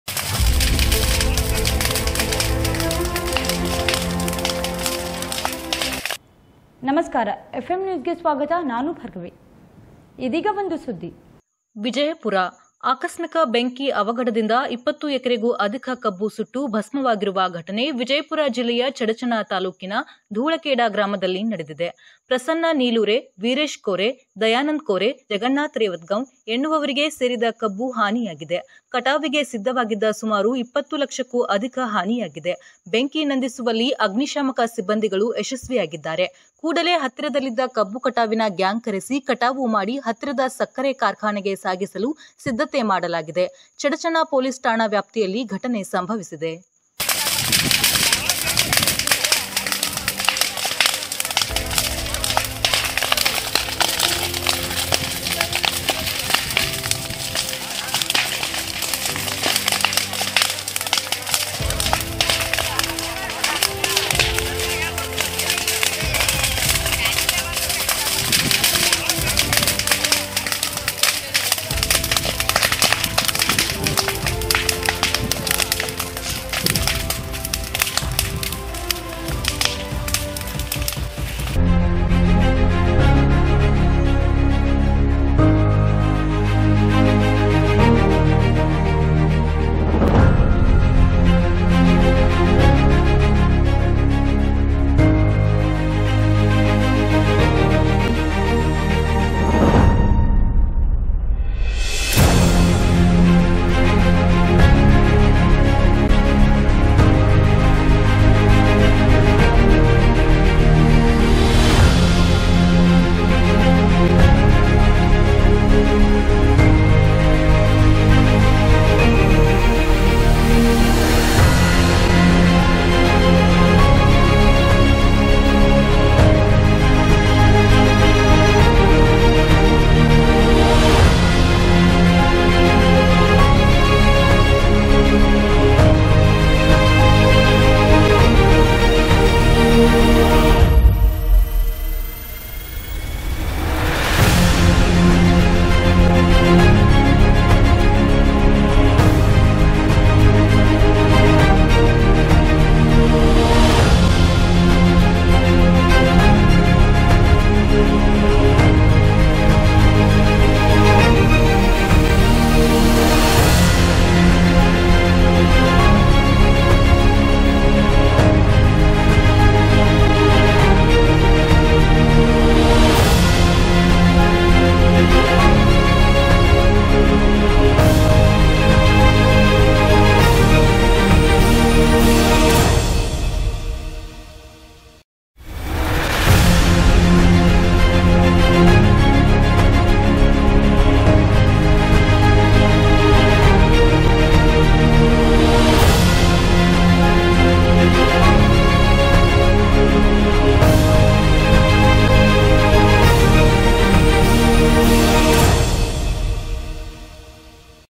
ಆಕಸ್ಮಿಕ ಬೆಂಕಿ ಸುಮಾರು 20 ಲಕ್ಷದ ಸಕ್ಕರೆ ಹಾನಿ | Fire accident | FM NEWS VIJAYAPUR
ಆಕಸ್ಮಿಕ ಬೆಂಕಿ ಅವಘಡ | Fire accident | ಆಕಸ್ಮಿಕ ಬೆಂಕಿ ಸುಮಾರು 20 ಲಕ್ಷದ ಸಕ್ಕರೆ ಹಾನಿ..!